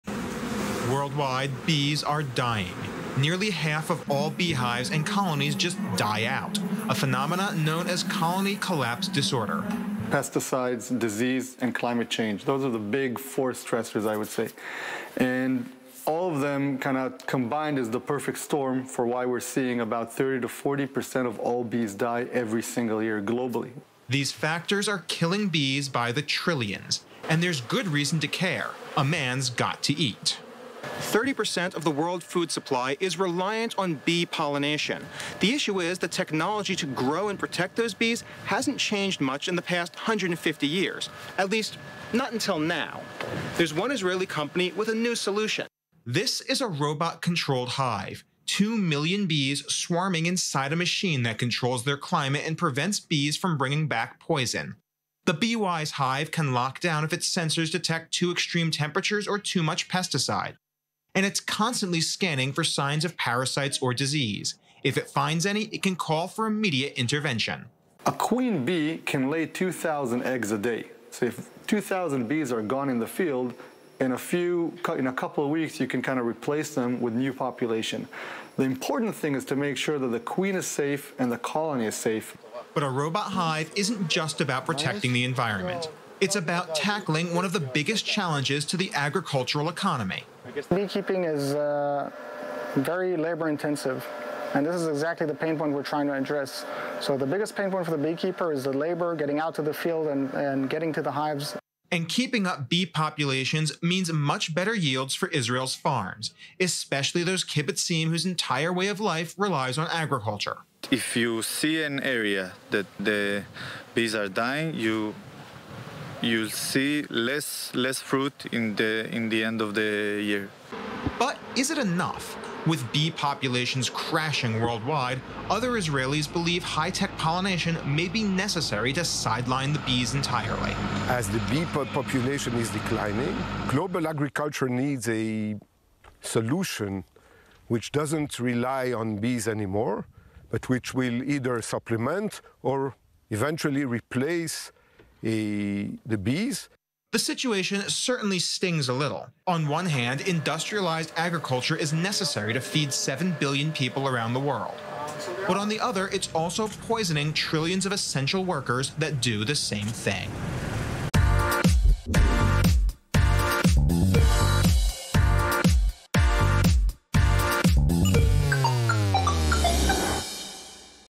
Israel's Robot-Controlled Beehive: Keeping Queen Bees and Colonies Alive
ISRAEL BUSINESS WEEKLY |